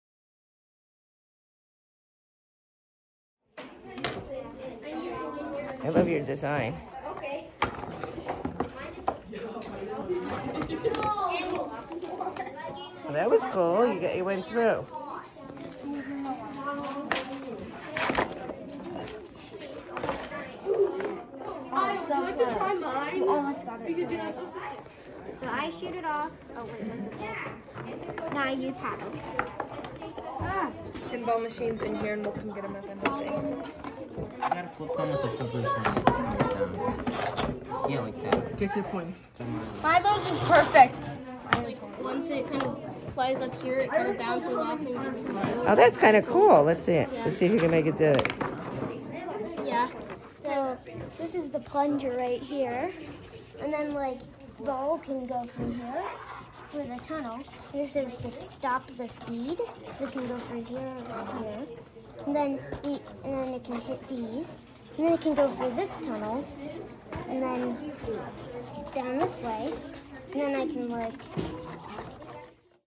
Pinball Machines Using a Pizza Box, Strawbees, Makedo, microbits
Pinball machines made by 3rd-6th grade campers.